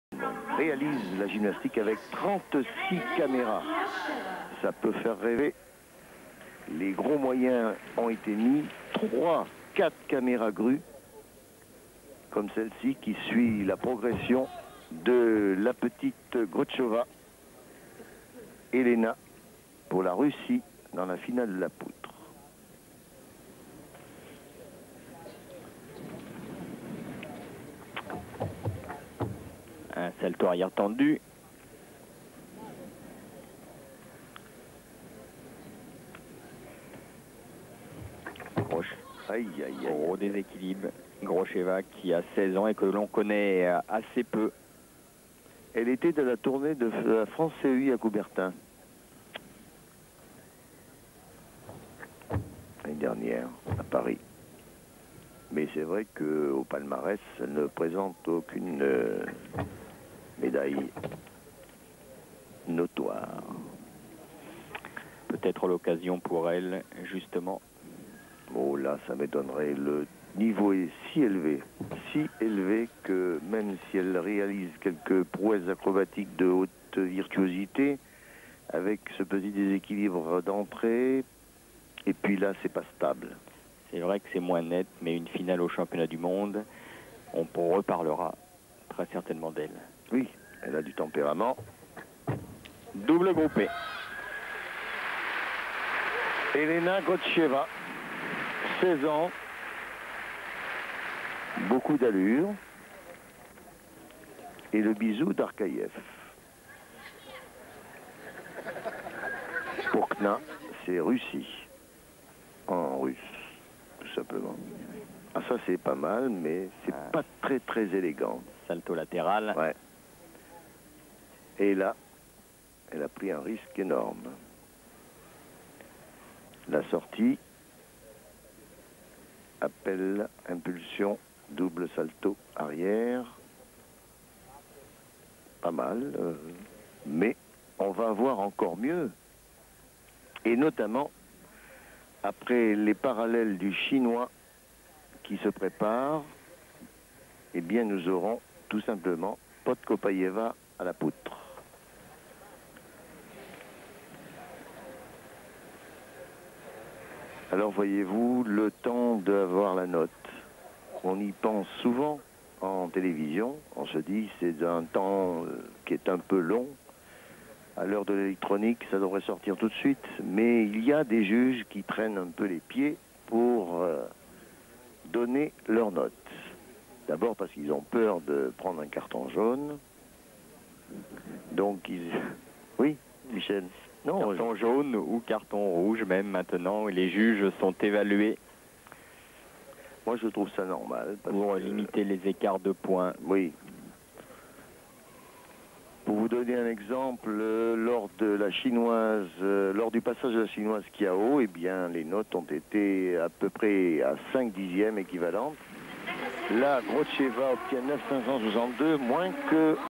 Elena GROSHEVA (RUS) beam - 1995 Sabae worlds EF
Elena GROSHEVA (RUS)

'95 World championships in Sabae - Event Final

9.562
Rank : 8th